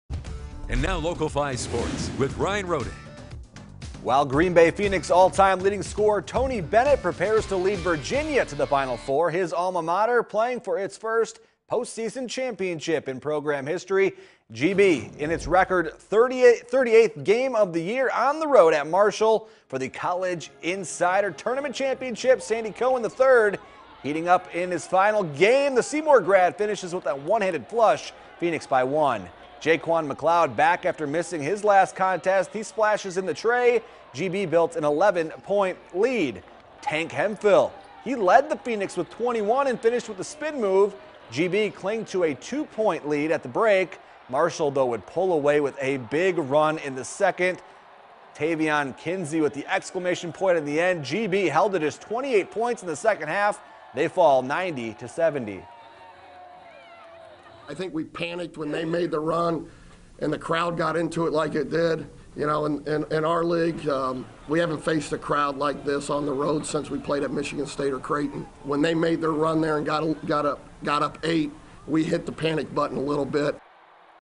Green Bay's Postseason Run Ends with Loss to Marshall
A big second half by Marshall proved to be the difference as the Green Bay Phoenix fell one game short of a CIT title on Thursday night.